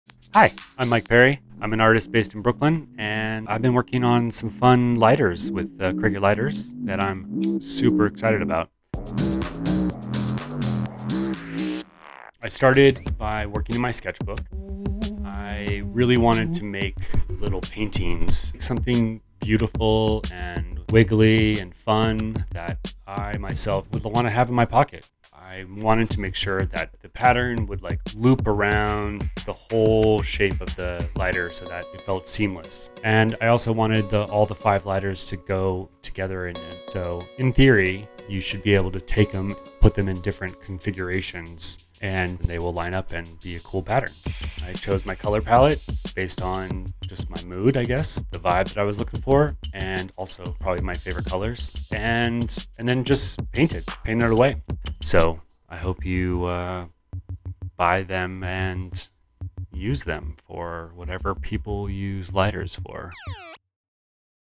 Mike Perry - Breaking the Grid // The Design Process
Mike Perry is connecting the lines. His pop culture artwork is bold, bright and beautiful. Brooklyn-based artist Mike Perry has created five different designs for Cricket lighters that all connect. We spoke to him about his sparks in life – and his fascination for fire rituals.
Learn about his design process when creating the Cricket collection "Breaking the Grid".